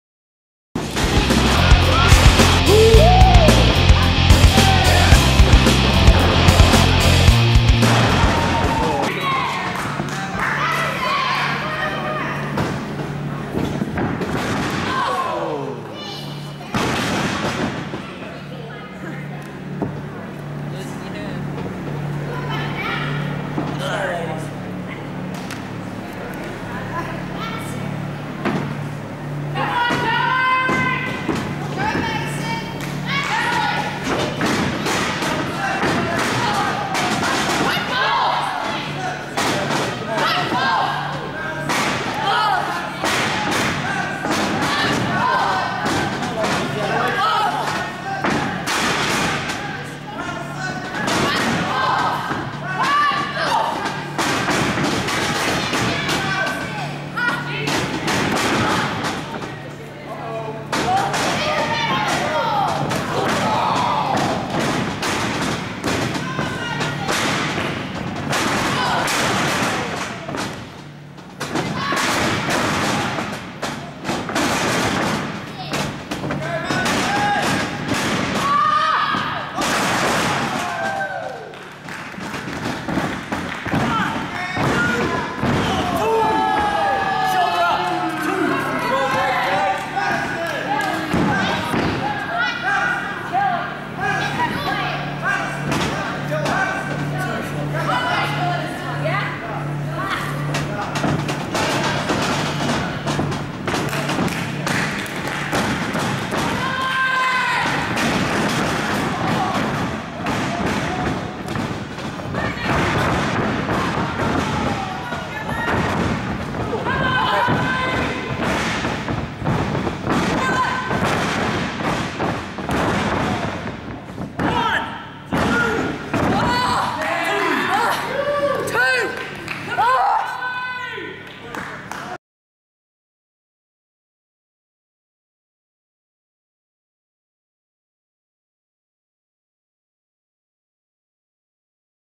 Madison Eagles vs Kellie Skater PREVIEW - Ignition 2013
A preview of the fantastic matchup between Madison Eagles and Kellie Skater that took place at IGNITION in 2013.

The Women of Pacific Pro Vol 1 DVD - COMING SOON